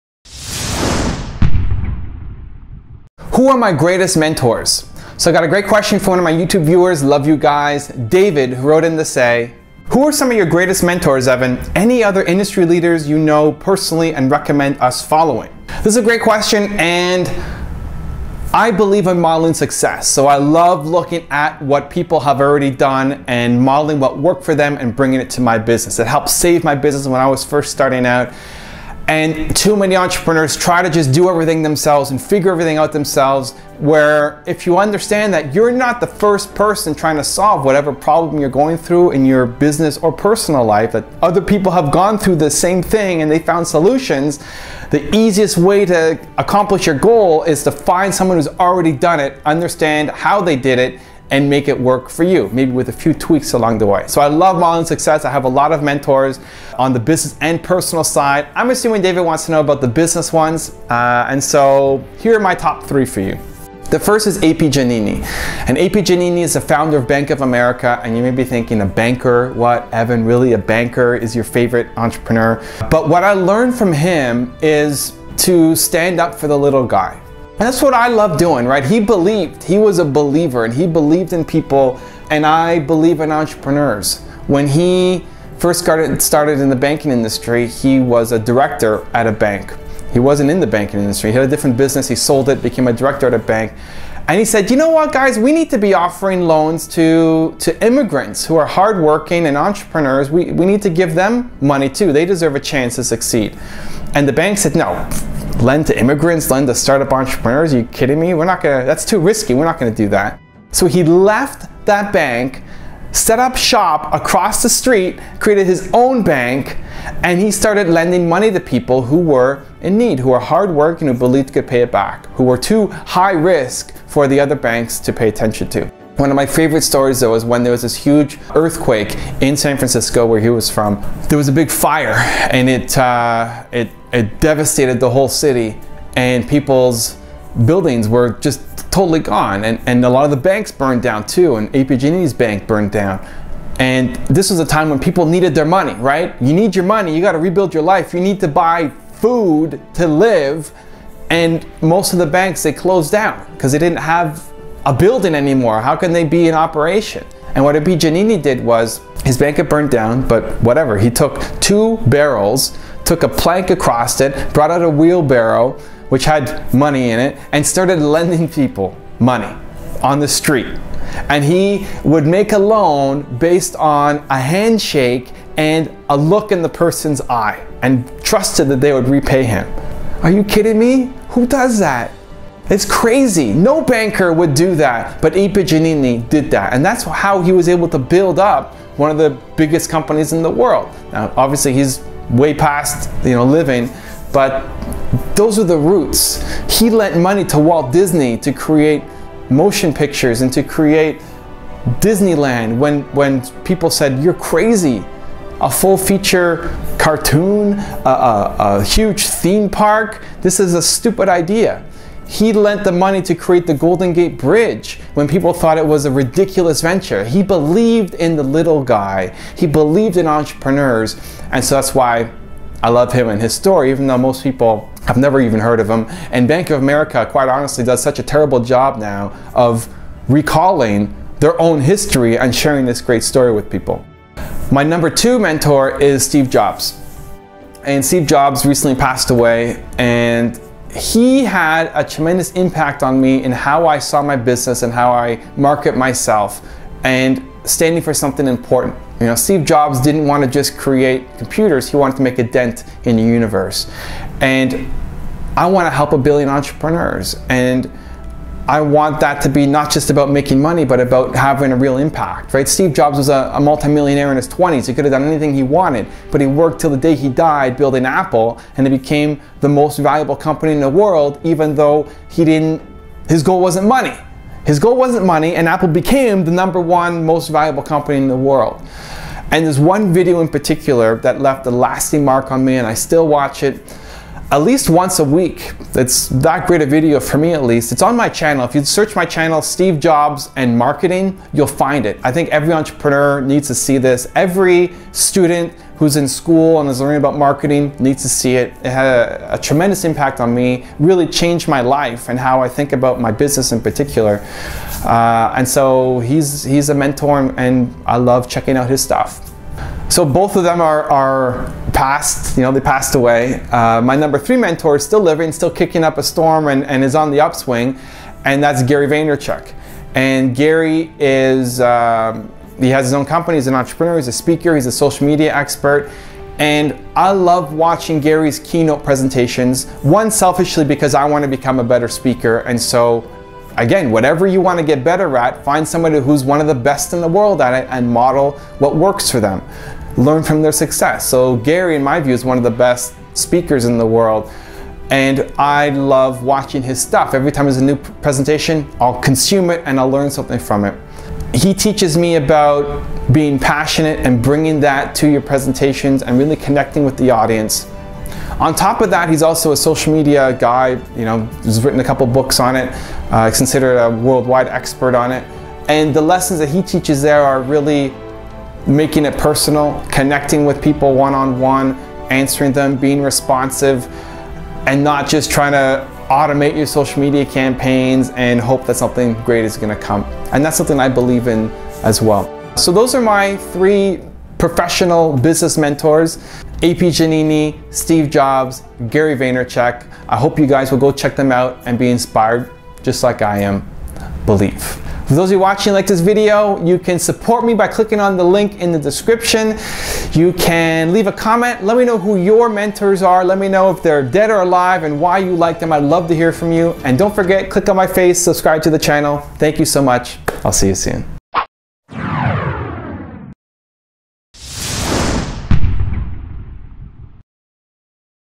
Mentorship Ideas - Who are my greatest mentors? - Steve Jobs, A.P. Giannini Success Stories
* Steve Jobs
* The Presentation Secrets of Steve Jobs
* Becoming Steve Jobs
* Steve Jobs: The Man Who Thought Differently
* Steve Jobs (Movie)

David:
"Who are some of your greatest mentors, Evan? Any other industry leaders you know personally and recommend us following?"